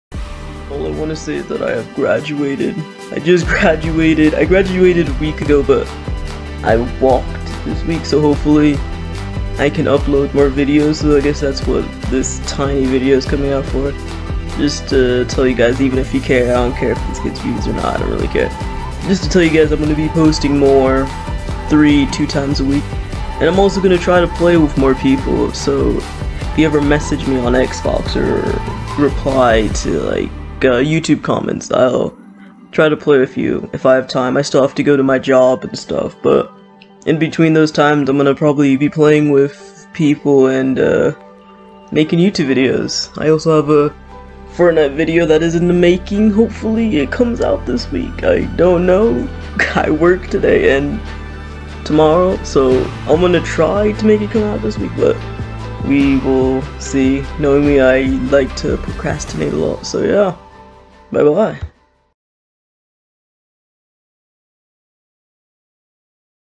I finally graduated!!
I'm gonna be posting more videos hopefully and playing with everyone I can. I want to give back to all my subscribers for staying so I'm gonna hopefully do a giveaway this summer.